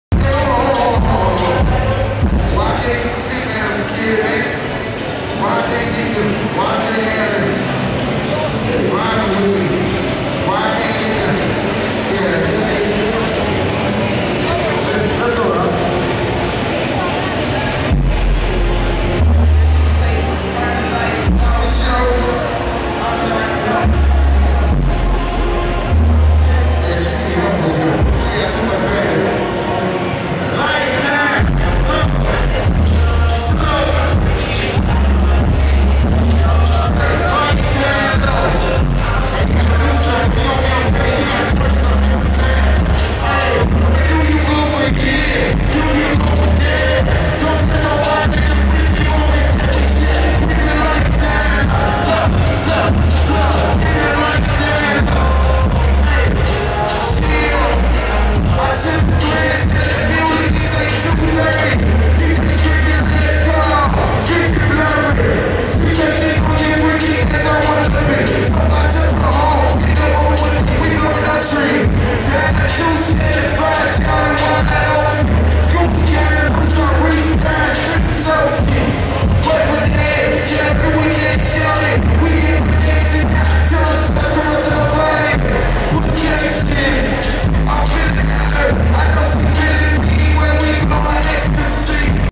Dub Car Show 2 Pistols Performing
2 Pistols Performing at 2009 Dub Car Show in Dallas Tx.